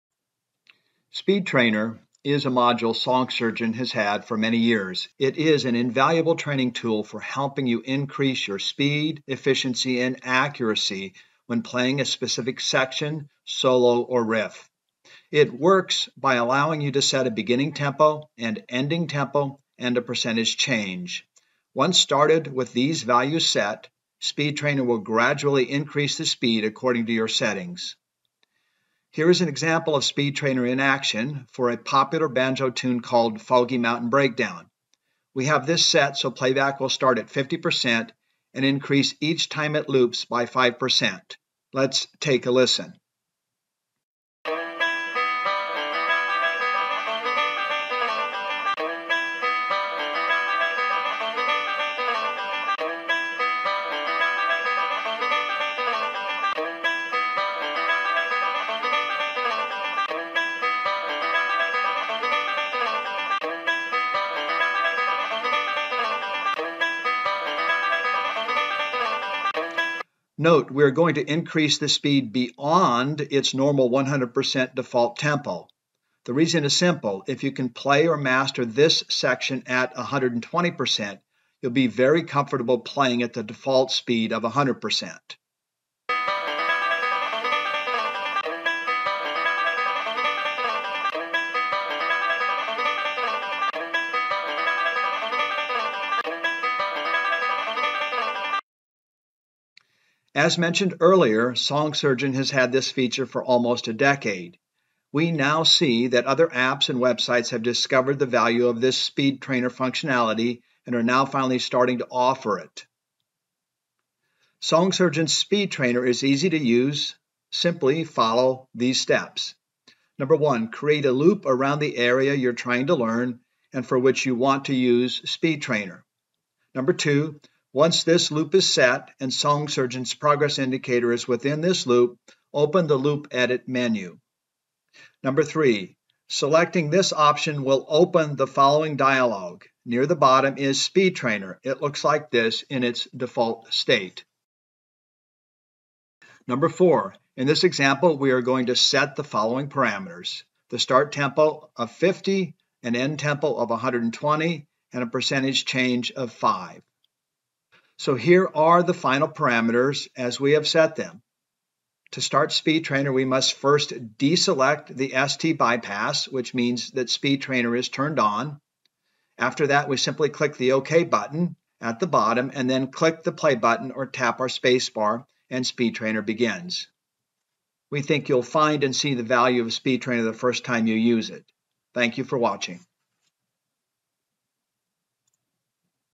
Song Surgeon 5: Speed Trainer demonstration
This weekly video tip ]] is a demonstration of Speed Trainer - a function found within Song Surgeon.